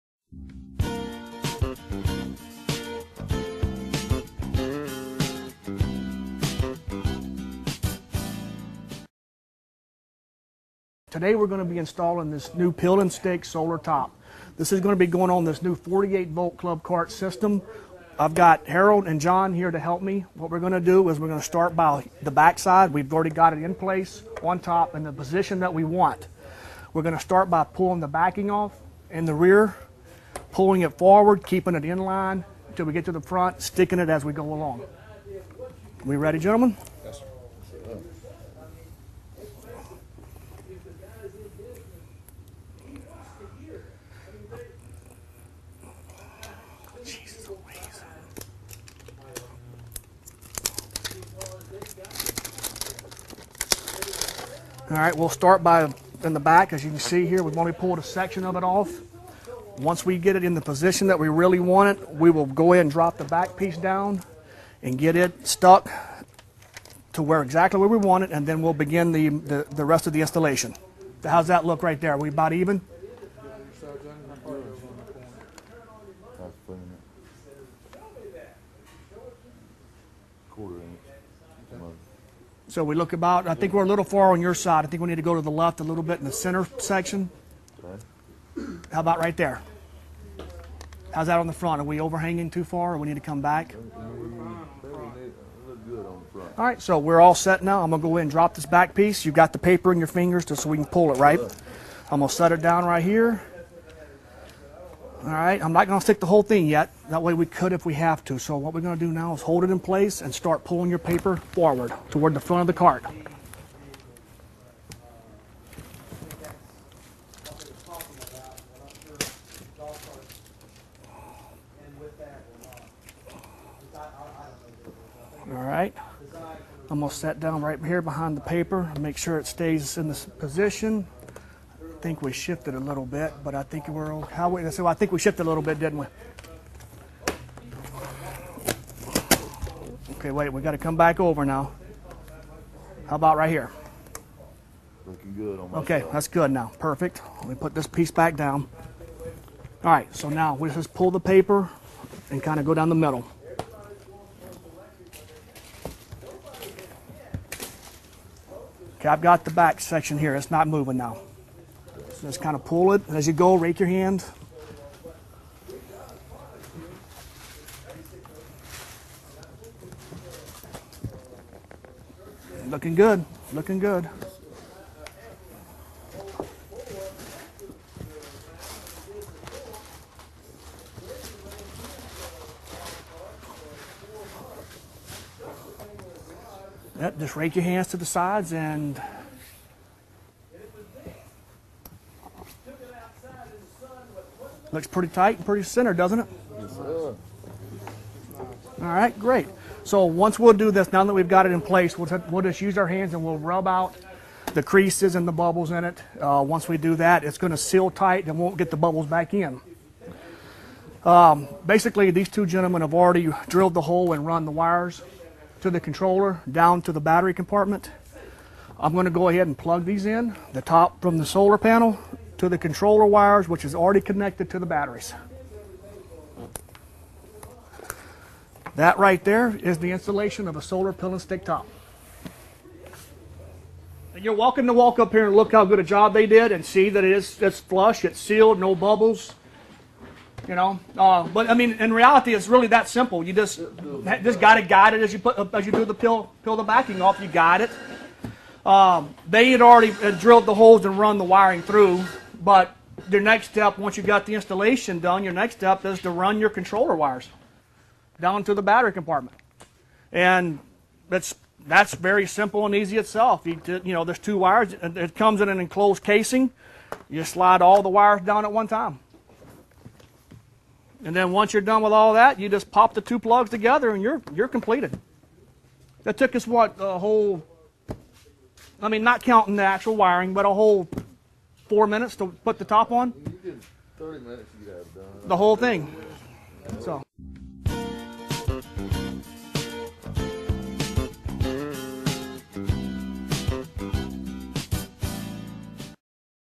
Earth Care Solar Golf Cart Top Installation Demonstration
Russell shows how quick and easy it is to install the Peel and Stick solar golf cart top. Only takes minutes to install and increases your battery life and extends travel time.